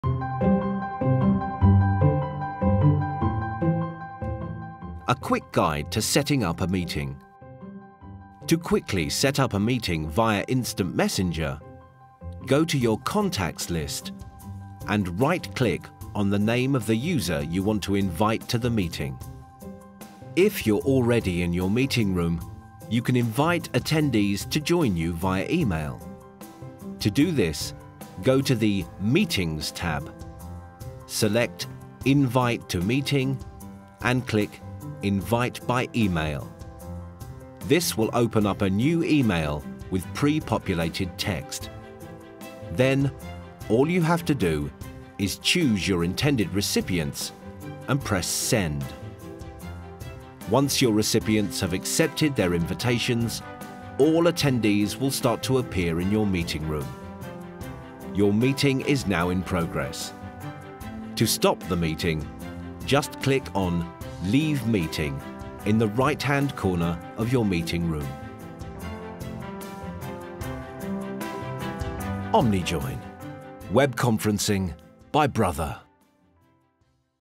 A quick guide to setting up a meeting
This is an OmniJoin tutorial video to help guide you through the product.